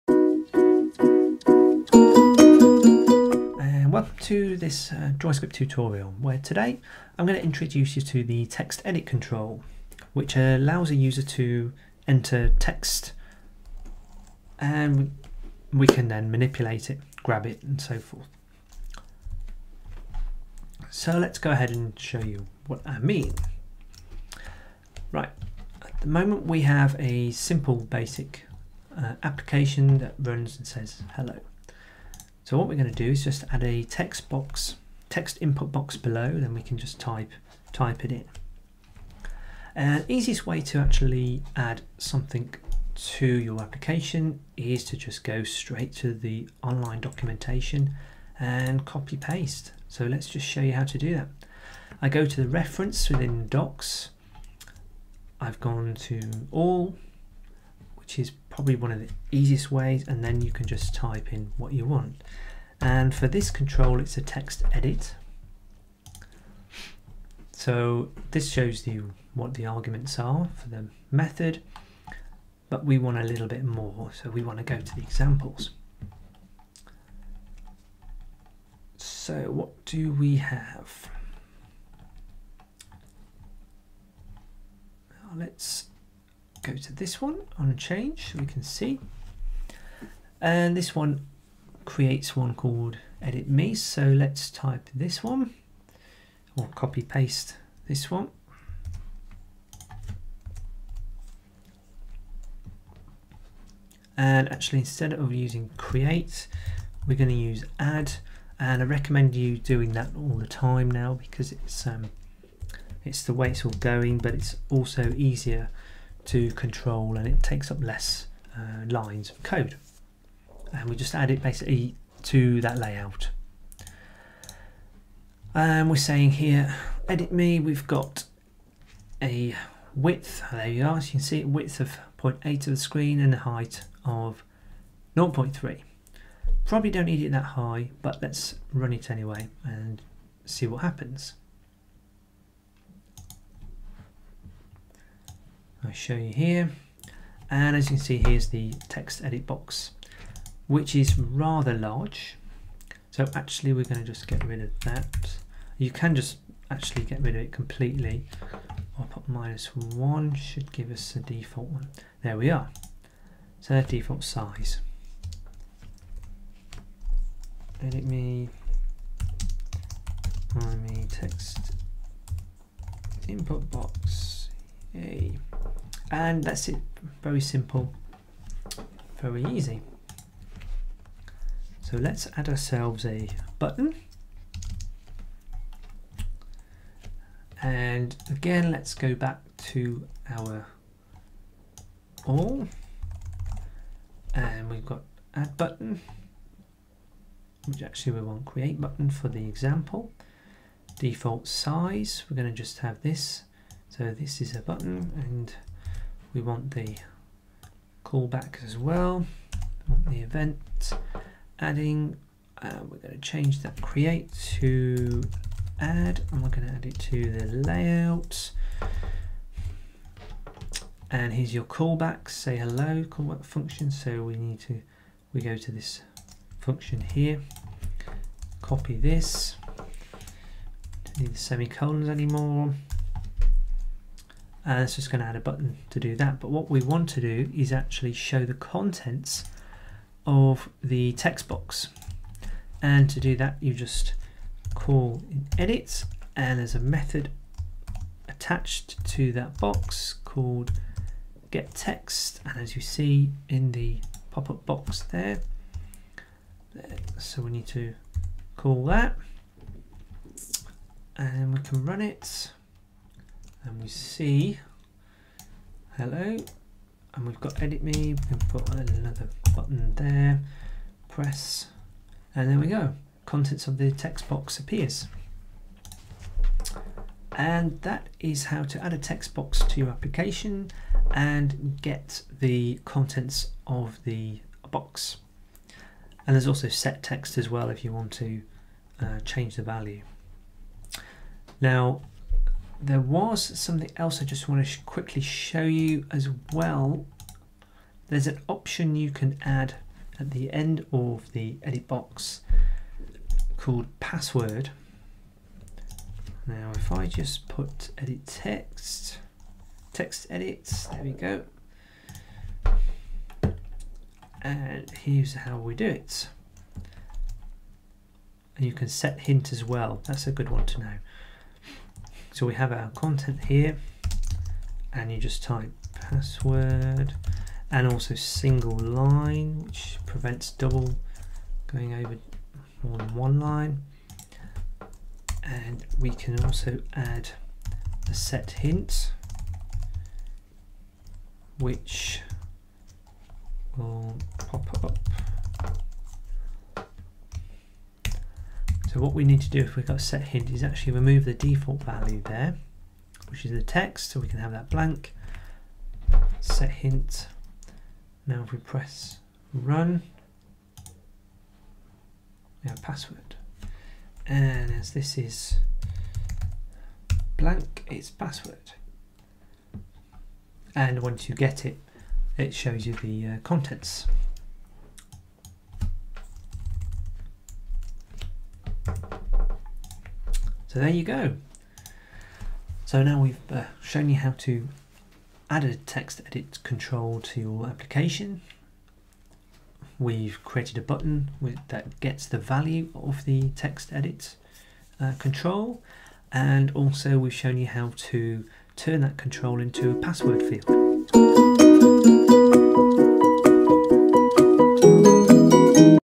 DroidScript Tutorials: Adding Text Input Elements
This video explains how to add text input elements to your droidscript apps.  The tutorial will show you how you can turn a text input element into a password field and also show you how you can get the contents of the input element when pressing a button.

For more online tutorials, please take a look at our tutorial